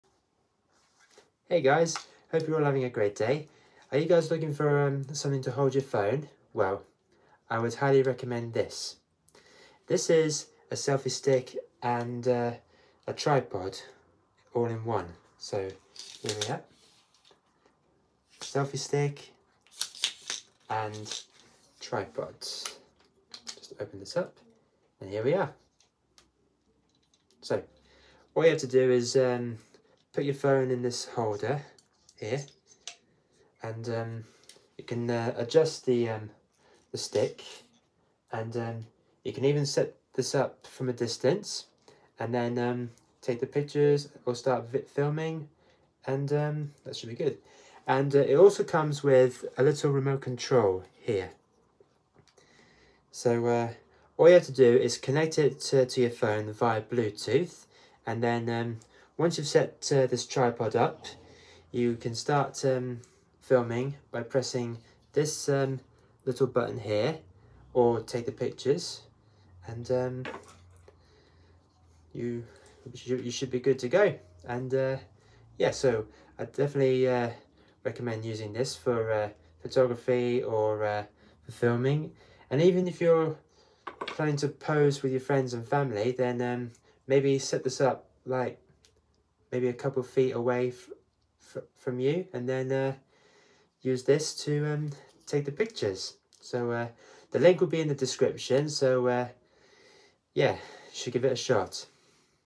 Selfie Stick Tripod recommendation
This phone holder is perfect for photography and video recording. A selfie stick and tripod, all in one. It comes with a little remote control that you can use to take pictures and start recording. All you have to do is link the remote to your smartphone via Bluetooth.